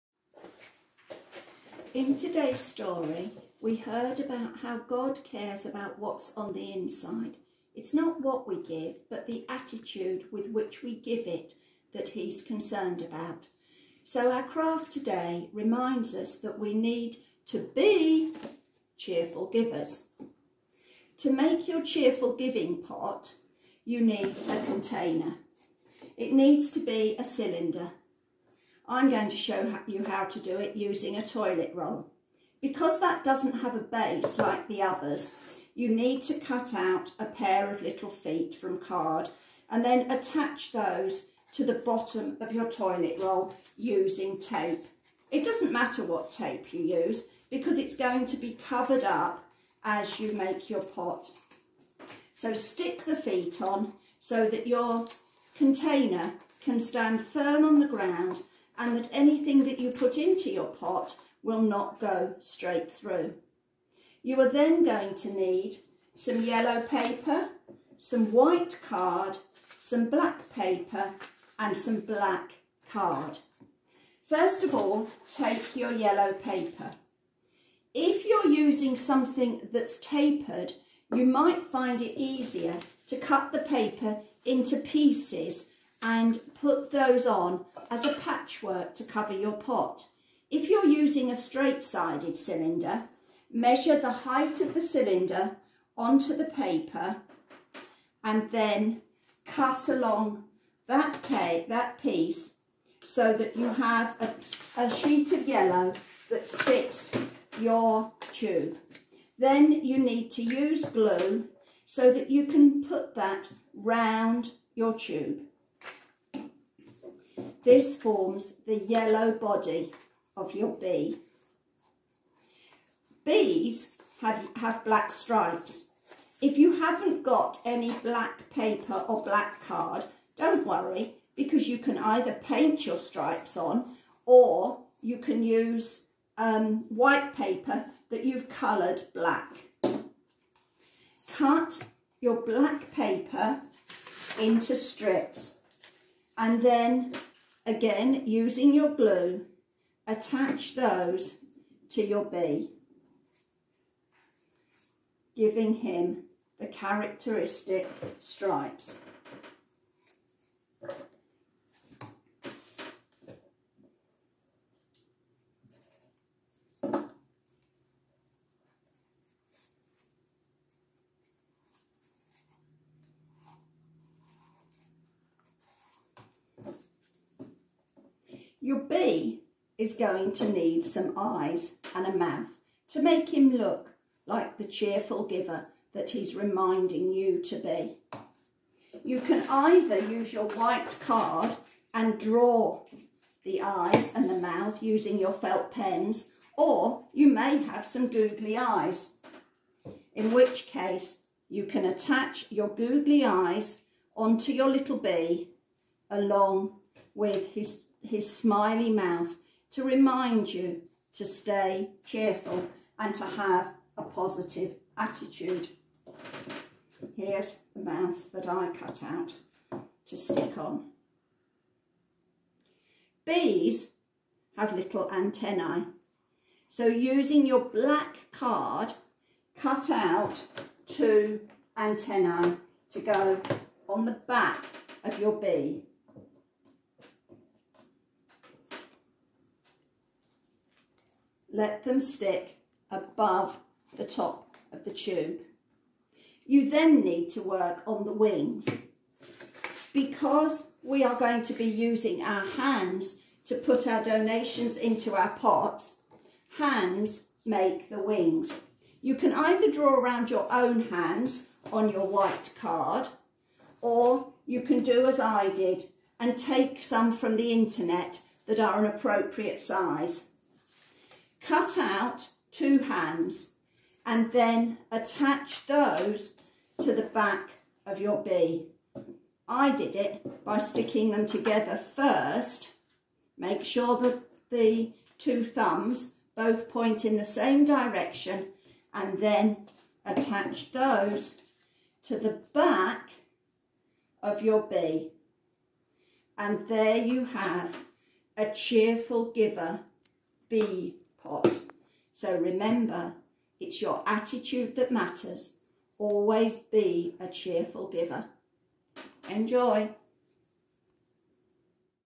CAYOS At Home - Craft Ep18 - Cheerful Giving Pots
CAYOS At Home

CAYOS Craft - Episode 18 - Cheerful Giving Pots
Today Sally shows you how to make cheerful giving pots to remind us that God is more concerned with our attitude to giving than what we give.
'Man looks at the outside but God looks at the heart' - 1 Samuel 16 v 7